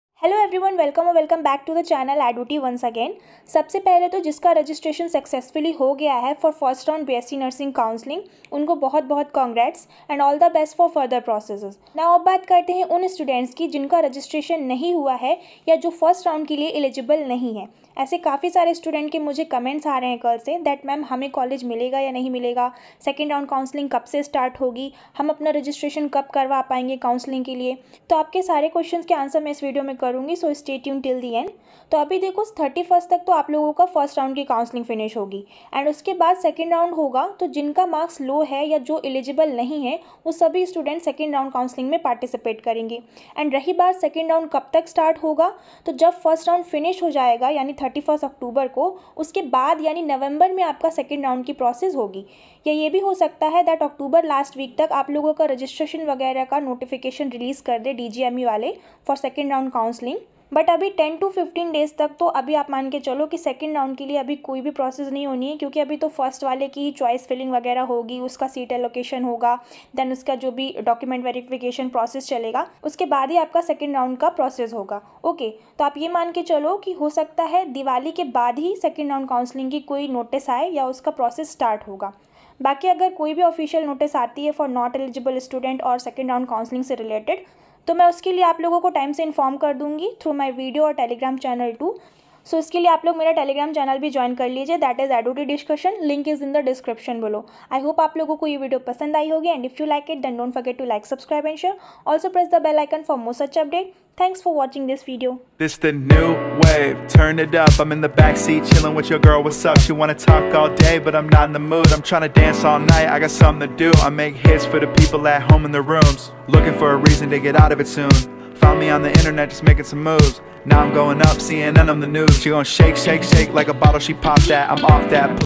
KGMU BSc Nursing Counselling 2022|| UP Bsc Nursing|| Abvmu BSc Nursing Counselling 2022
Hey Guys! welcome to the channel Edutyee. Here in this video I just shared the expected dates for 2nd round Counselling of Up bsc nursing 2022 for those who are not eligible for 1st round Counselling.

Stay motivated stay preparing 🤗